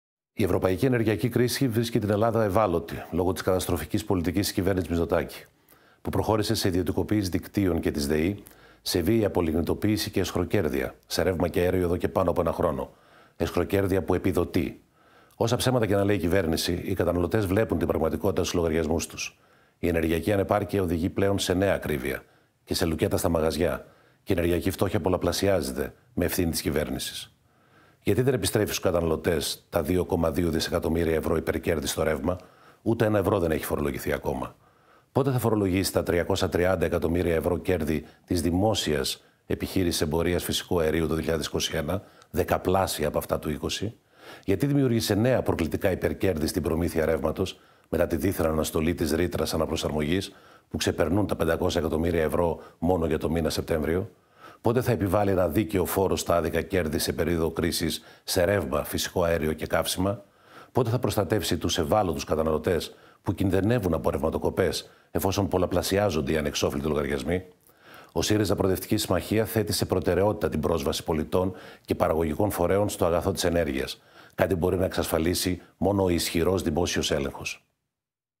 Σ. Φάμελλος: Τα ψέματα της κυβέρνησης Μητσοτάκη δεν μπορούν να κρύψουν ακρίβεια και αισχροκέρδεια
Δήλωση (on camera) του τομεάρχη Ενέργειας και Περιβάλλοντος της ΚΟ του ΣΥΡΙΖΑ-Προοδευτική Συμμαχία, Σωκράτη Φάμελλου